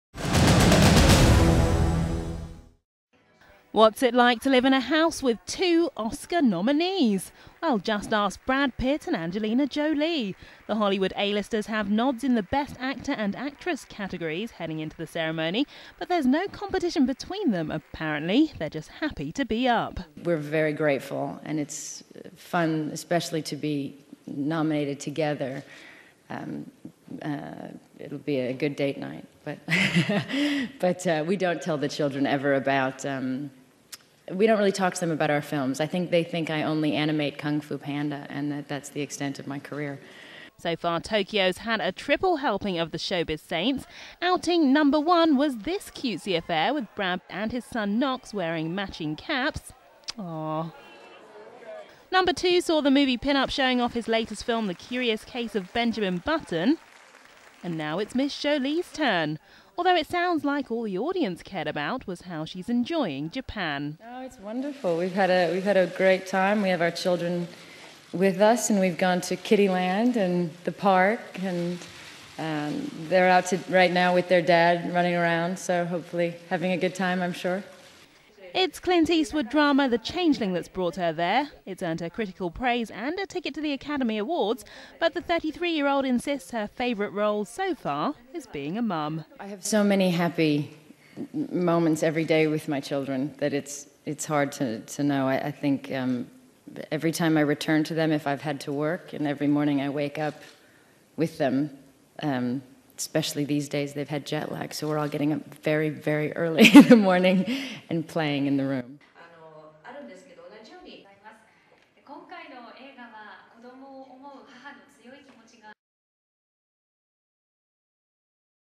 Angelina Jolie wows fans in Japan
Showbiz saint shows off her la film The Changeling in Tokyo and talks about being up for an Oscar along with Brad Pitt. Test